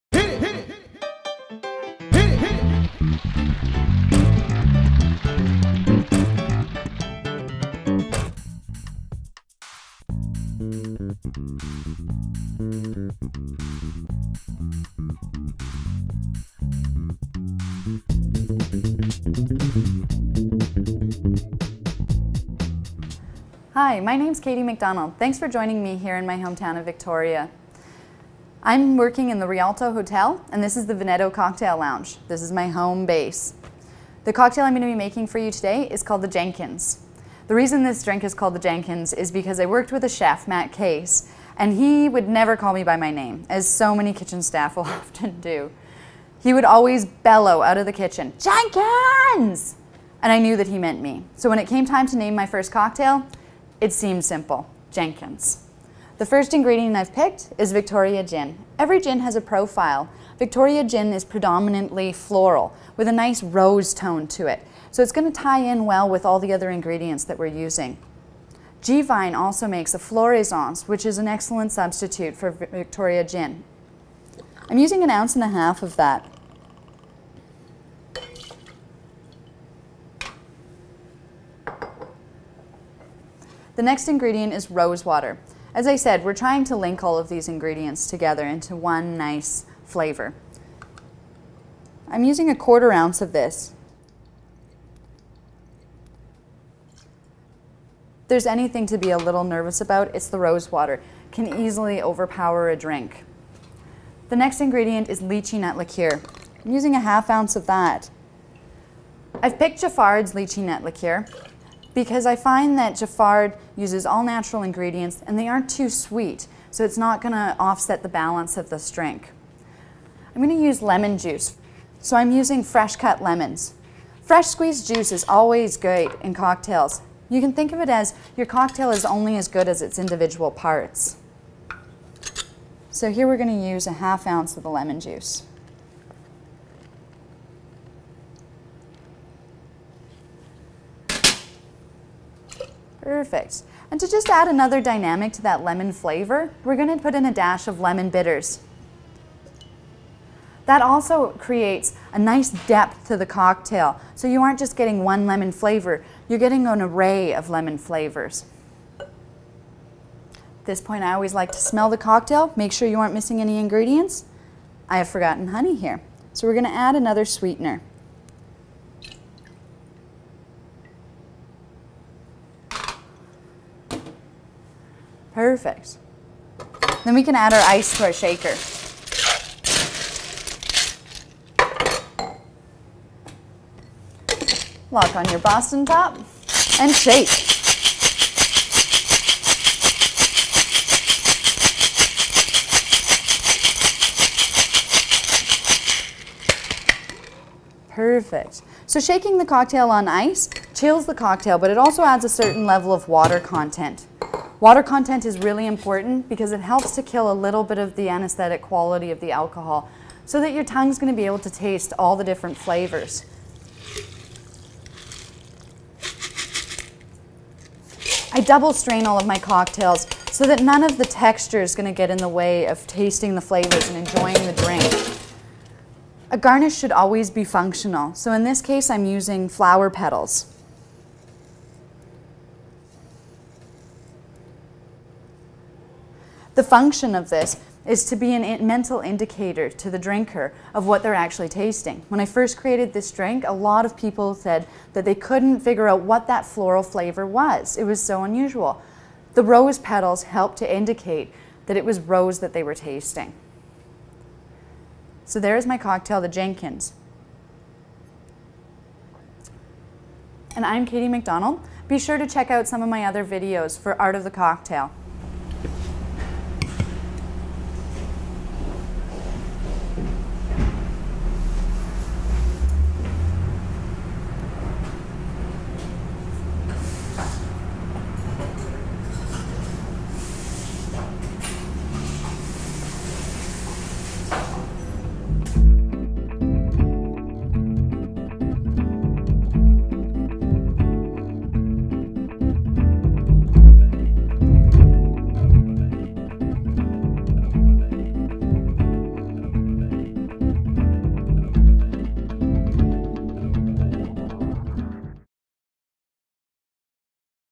Art of the Cocktail 2010 - How to Make a Jenkins Cocktail
MORE TO COME IN 2010 - Katie McDonald explains how to make one of her orignal cocktail  creations, the Jenkins, at Veneto Tapa Lounge in Victoria, BC, Canada for The Art of the Cocktail Festival.